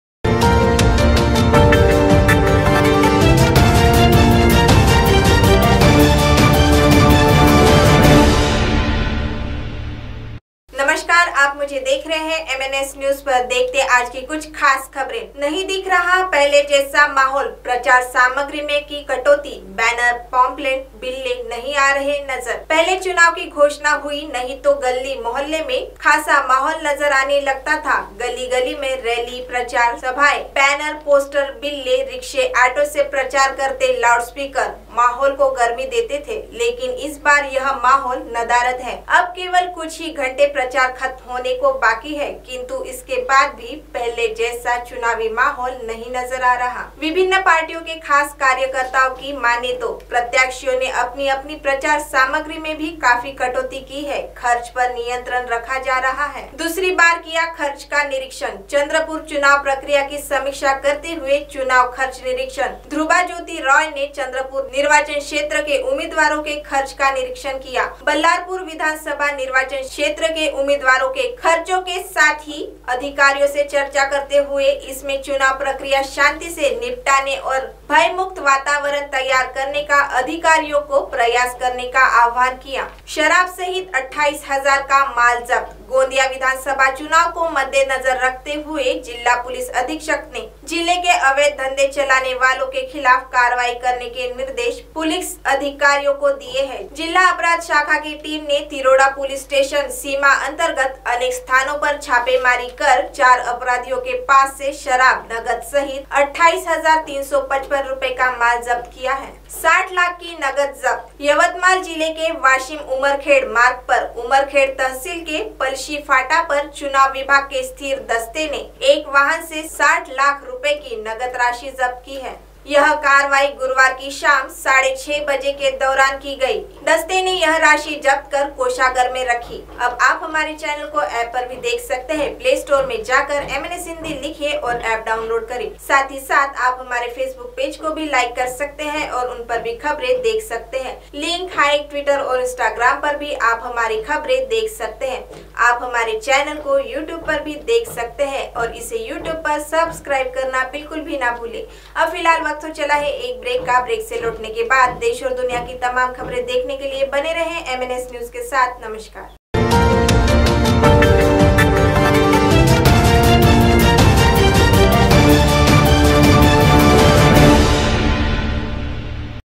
MNS गोंदिया-अवैध शराब सहित 28,000 का माल जब्त।
MNSNEWSLIVE गोंदिया-अवैध शराब सहित 28,000 का माल जब्त।